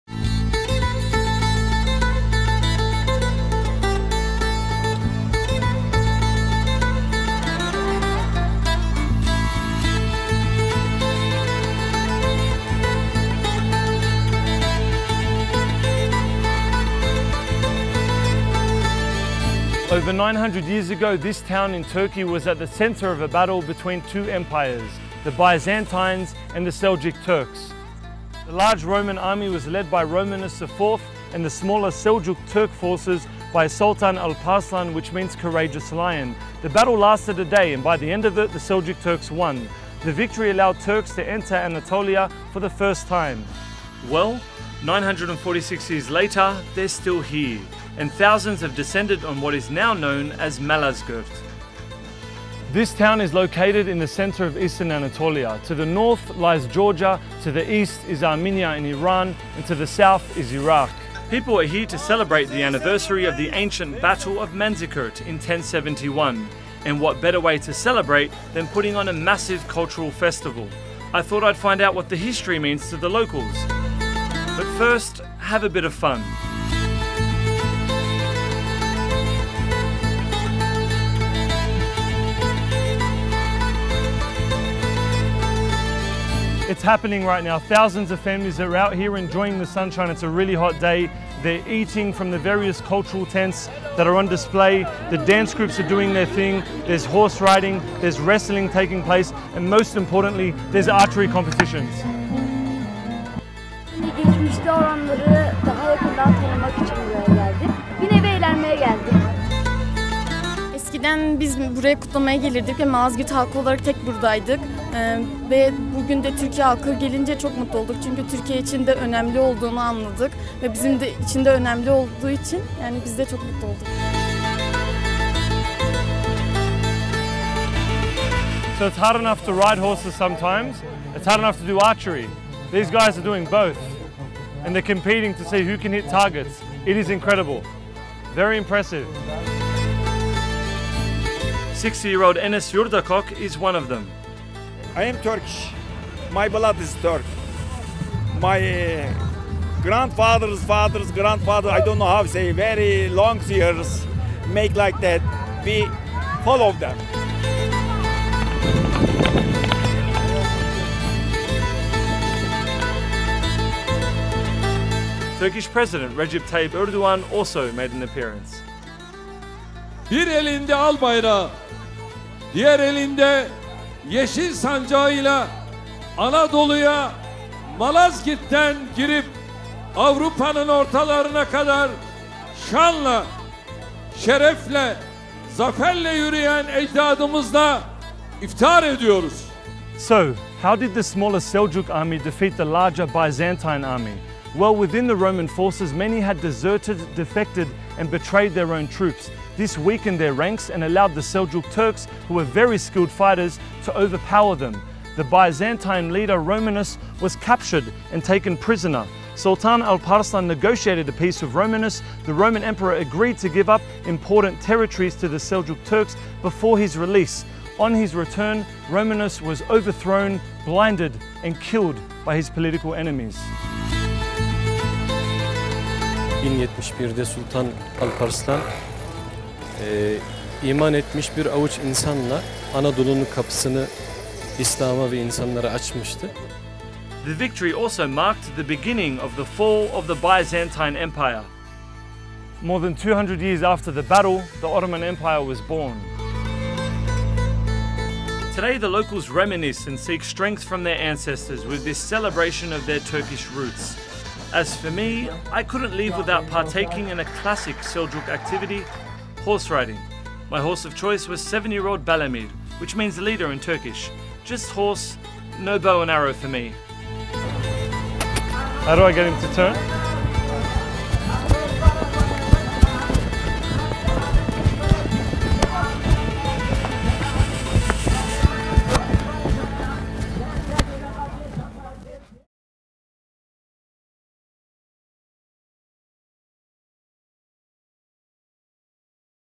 Battle of Manzikert: Turks' First Step into Anatolia
In 1071, the Seljuk Turks defeated the Byzantines in the Battle of Manzikert and opened up Anatolia for Turkish rule. We visited Malazgirt in eastern Turkey to find out what the history means.